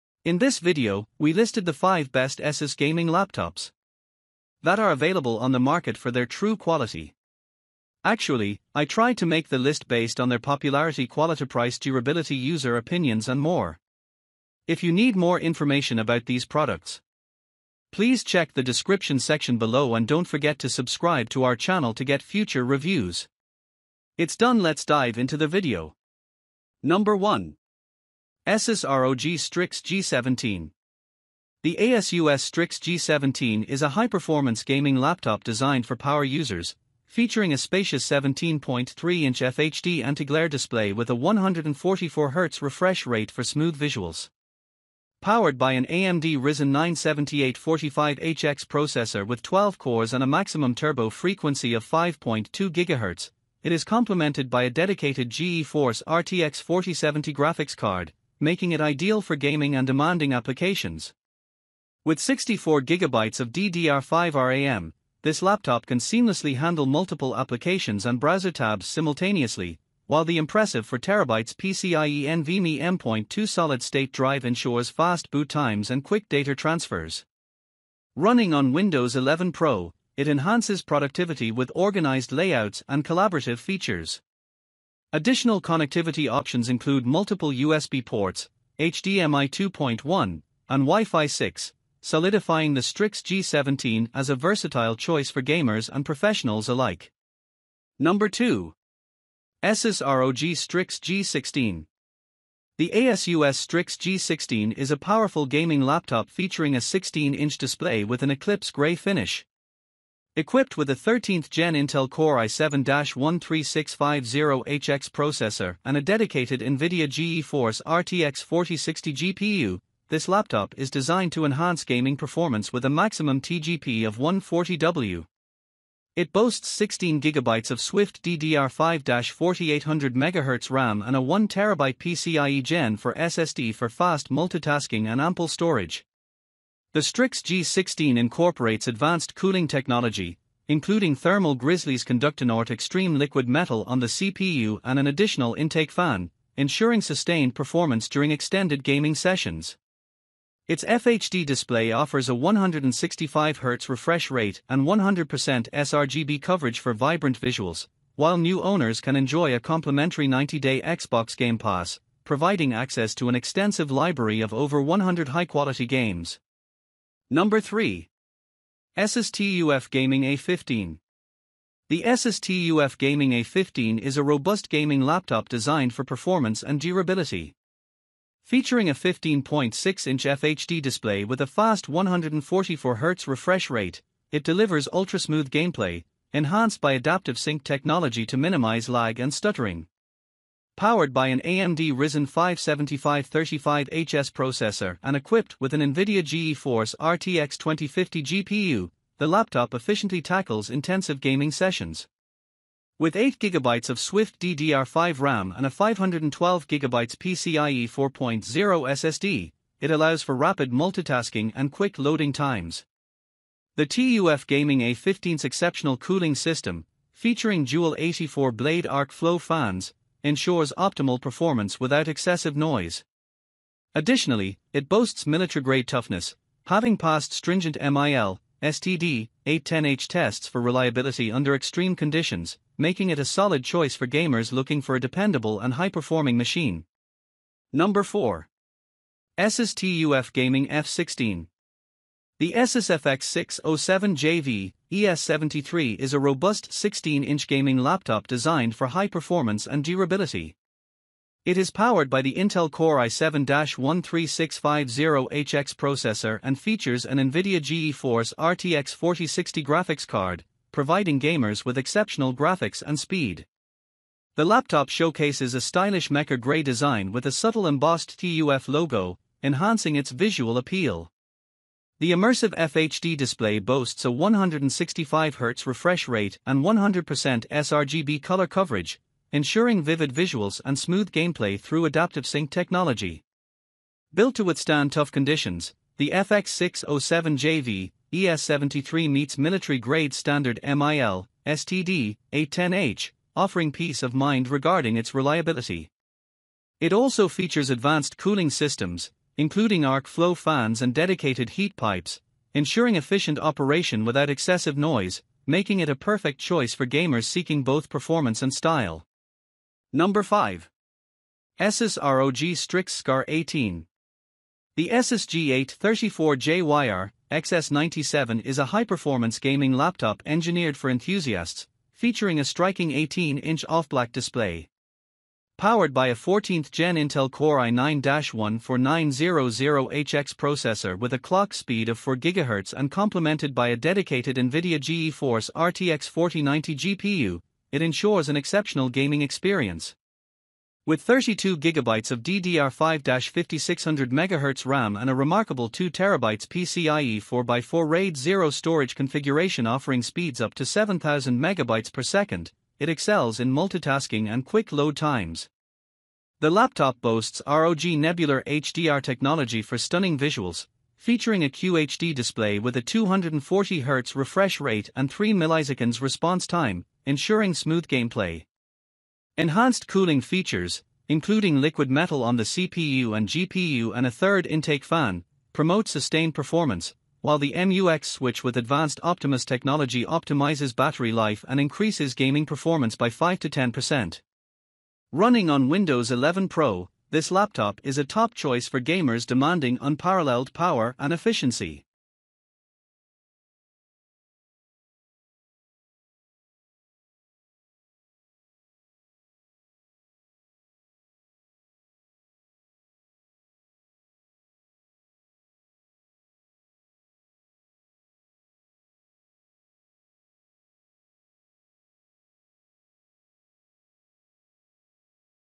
5 Best Asus gaming laptops | A guide to buying an Asus gaming laptop
5 Best Asus gaming laptops | A guide to buying an Asus gaming laptop

🌟 Welcome to our ultimate guide on the Best Asus Gaming Laptops! 🎮 Are you on the hunt for the perfect gaming rig that combines cutting-edge performance with sleek design? Look no further! In this video, we’ll dive deep into the top Asus gaming laptops available in 2023, breaking down their standout features, performance benchmarks, and how they can elevate your gaming experience. Whether you're a casual gamer or a hardcore enthusiast, we’ve got something for everyone. 💻✨

      owners.